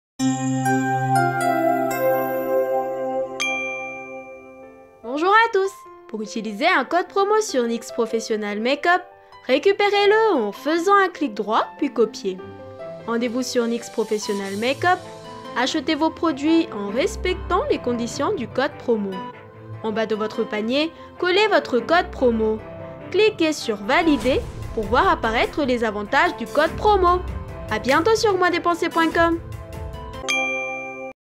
Comment utiliser un code promo Nyx Professional Makeup
Utilisation en vidéo d'un code promo Nyx Professional Makeup à récupérer sur Moins-Depenser